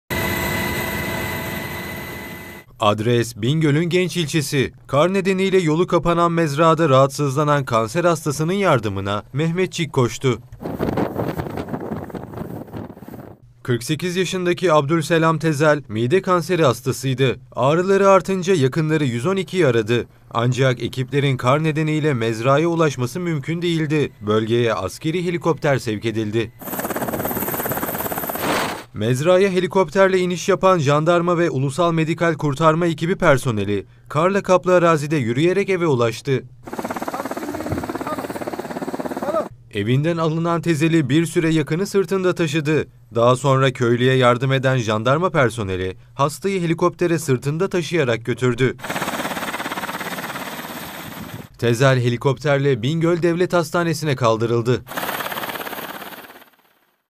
Hastanın yardımına Mehmetçik koştu
Kar nedeniyle yolu kapalı mezrada rahatsızlanan kanser hastasının yardımına Mehmetçik koştu. Bingöl'de yoğun kar yağışı nedeni ile sağlık ekiplerinin mezraya ulaşması mümkün olmayınca yardıma askeri helikopter sevk edildi. Jandarma personeli hastayı helikoptere sırtında taşıyarak götürdü.